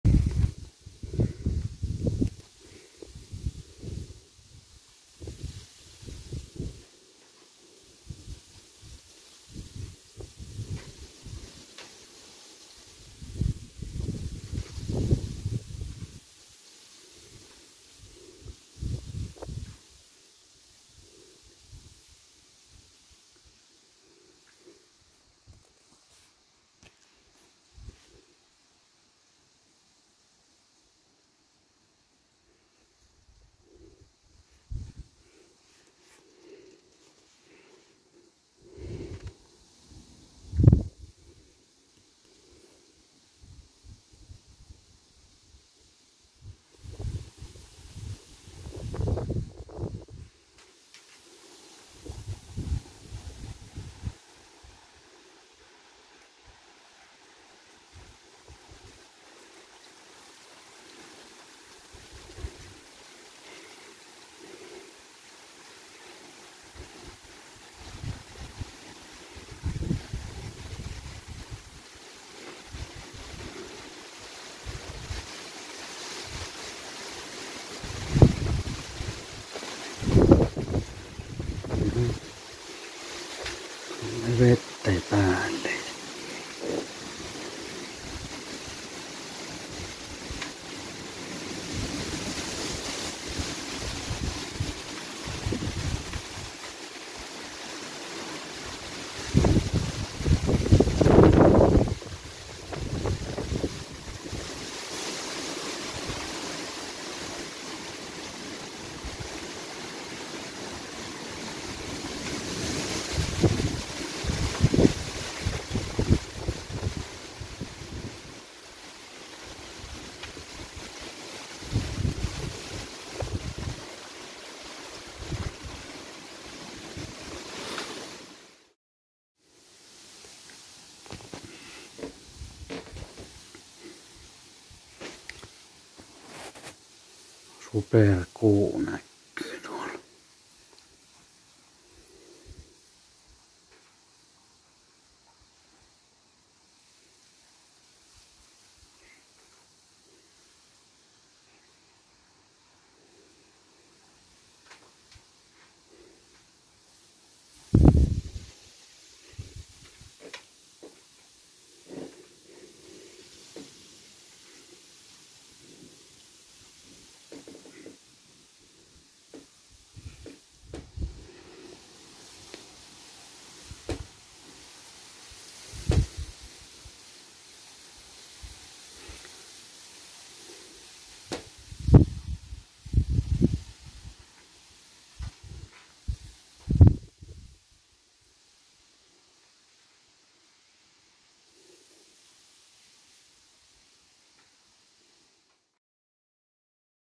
Ukkonen 2024⛈🤔❓ & Superkuu 19.8.2024
Video litteri😏:
Ukkospilvi: Kyllä
Ukkostaako: Ei, virtaloppu kesken 🤭
Satoiko: Satoi🌧🌦
Paistoiko aurinko: Paistoi🌦☀️🥵
Näkyikö sateenkaari🌈: Näkyi, mutta en kuvannu🥴
Näkyikö/Paistoiko Superkuu: Kyllä näky, mutta ei paistanu.🥶😆🙂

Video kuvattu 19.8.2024